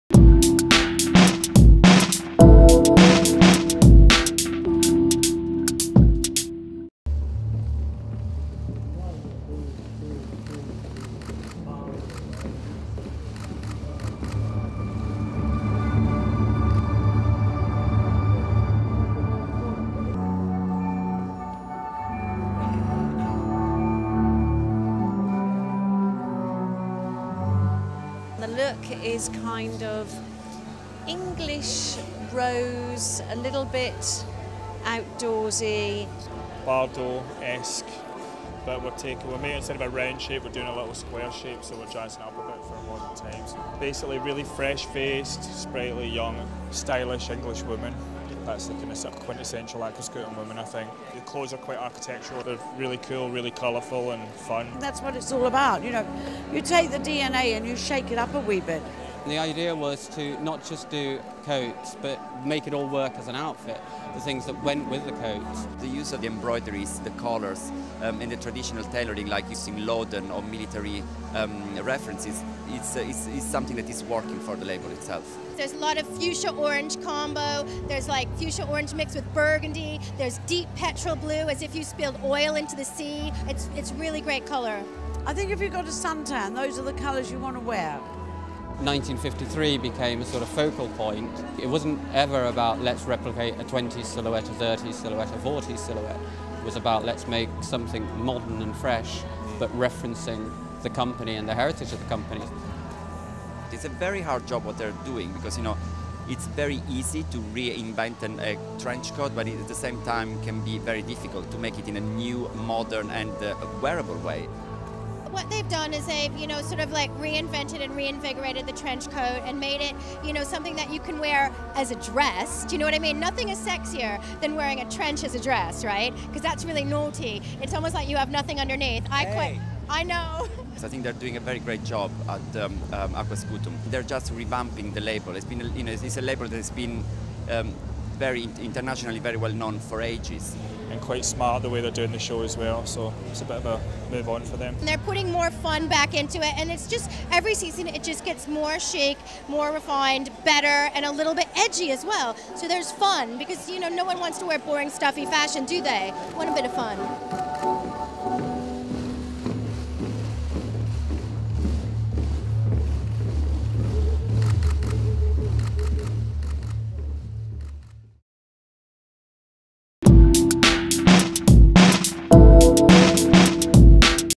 Fashion Show - Aquascutum: Fall 2008 Ready-to-Wear
Runway, backstage, and front-row footage from the London show.  Visit Style.com for more runway shows, fashion trends, shopping guides, and news about models and designers.

Starring: Joanna Sykes